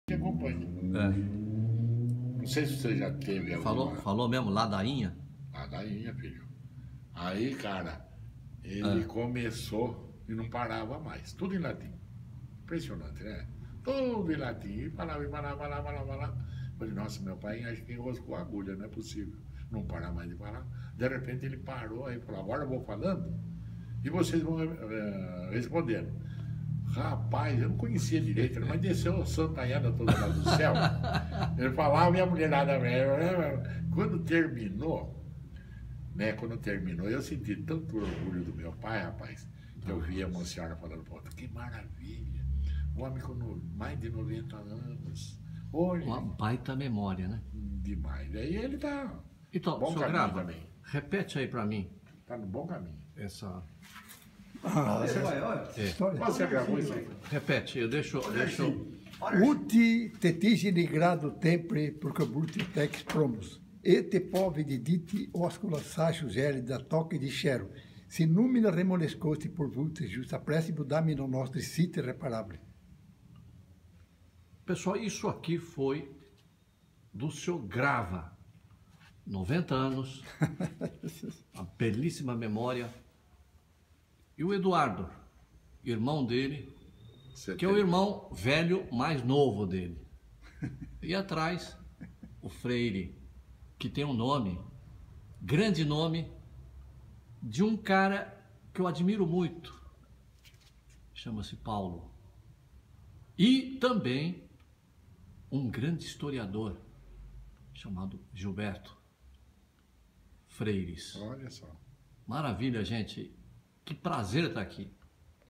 EDUARDO E ANERCIO MARCO GRAVA : BOM HUMOR ATÉ EM LATIM
serie Memorias  2018, mar 11,  São Manuel SP
Em Sao Manuel interior de Sao Paulo, ma  conversa bem humorada em latim e num bom português com Eduardo Grava e seu irmão, ANERCIO MARCO GRAVA que aos noventa anos,  esbanja humor, memória e vitalidade e  espiritualidade raros.